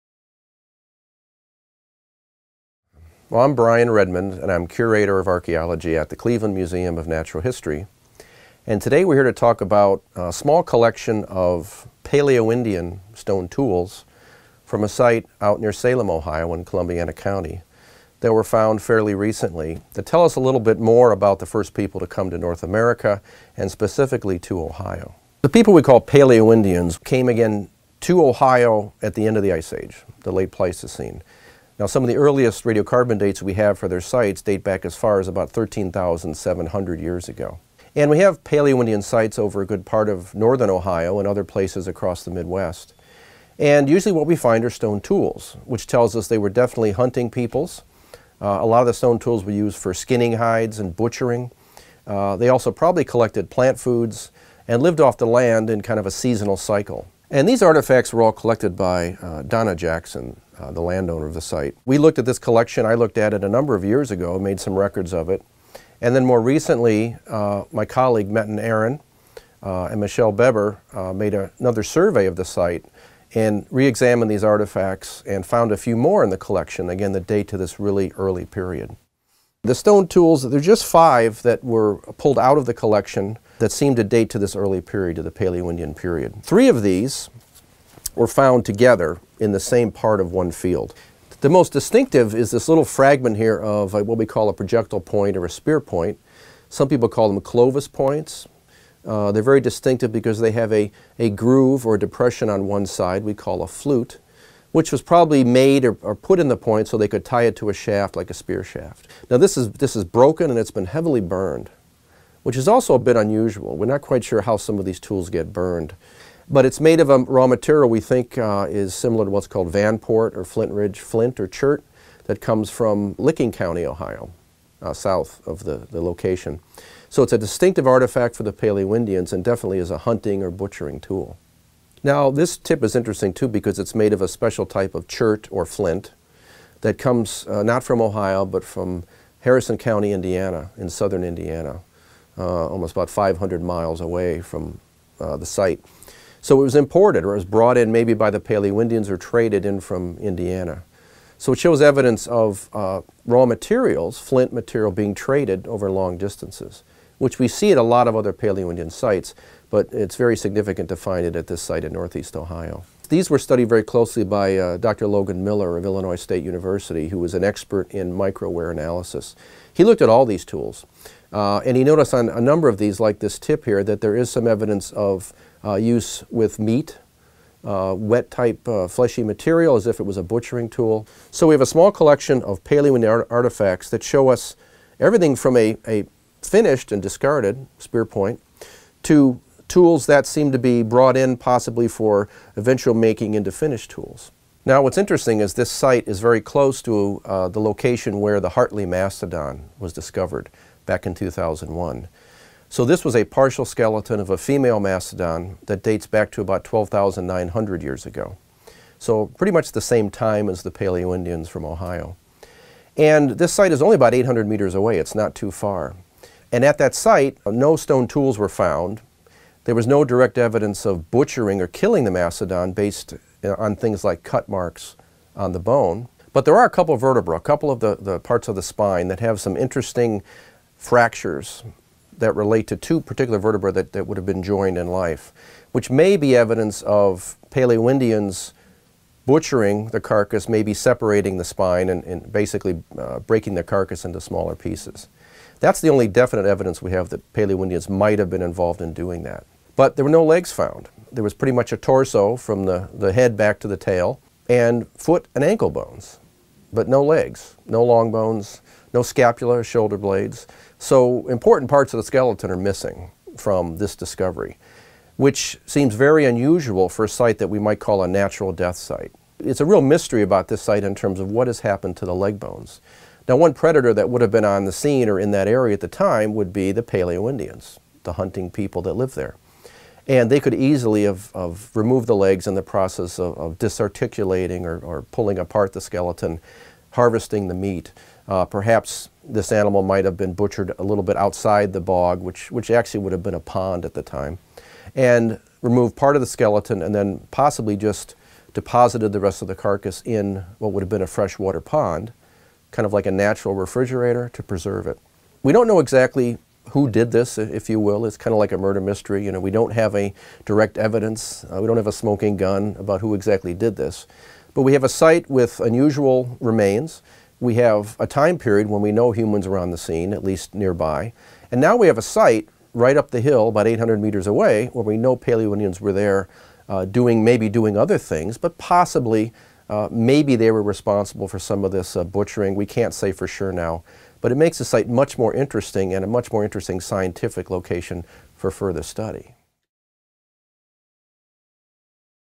Hartley Mastodon Mystery Clues
Ancient Stone Hunting Tools May Be Related to Animal's Demise:

A collection of five Paleoindian tools found near where the Cleveland Museum of Natural History Museum’s Hartley mastodon skeleton was unearthed in eastern Ohio shows that ancient hunters were spending time in the area in roughly the same time period when the animal died 13,000 years ago.

In this video, the Museum’s Curator and John Otis Hower Chair of Archaeology, Dr. Brian Redmond, explains the tools’ discovery, what analysis reveals about them, and how they might be connected to the mastodon. The design of the five tools recovered from a Columbiana County farm indicates that they were produced by prehistoric Native Americans from the Clovis culture, which occupied Ohio and other North American sites at the end of the last ice age.

Dr. Redmond and colleagues from Kent State University, Illinois State University, Southern Methodist University and Rogers State University recently published the results of their analysis of the tools in the Journal of Archaeological Science: Reports.